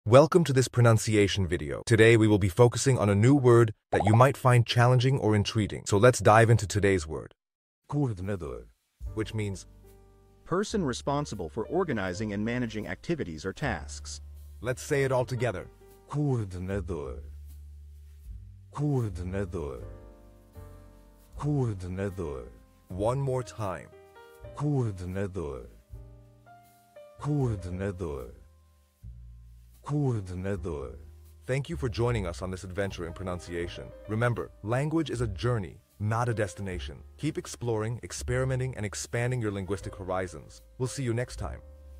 How to pronounce Coordenador
Master the Pronunciation of 'Coordenador Which Means Coordinator' - which means : Person Responsible for Organizing and Managing Activities or Tasks. 📝 with @PortuguesePronunciation 🎓🗣️ - [Your Guide to Portuguese]

Learn to pronounce 'Coordenador' perfectly in Portuguese with our easy and engaging tutorial by @PortuguesePronunciation! 📖📽️ In this comprehensive video, we provide you with native speaker examples 🗣️👂 and phonetic breakdowns 🎵 to ensure you get the pronunciation just right.

How to say Coordinator in Portuguese